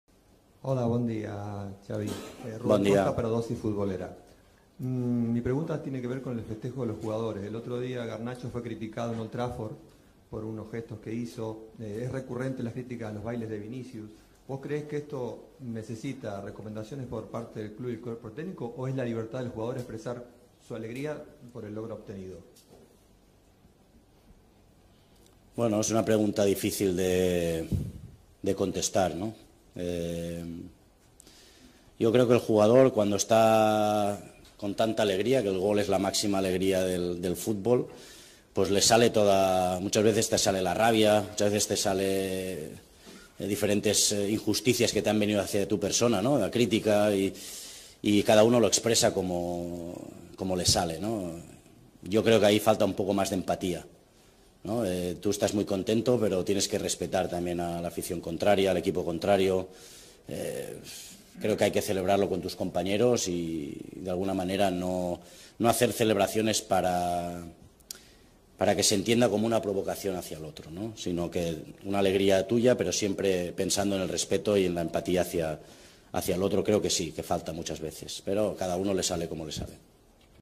BARCELONA | XAVI y las celebraciones de VINICIUS y GARNACHO: significativa la respuesta| AS
El entrenador del Barcelona fue preguntado por esta cuestión en rueda prensa previa al duelo contra el Almería. No se mordió al lengua.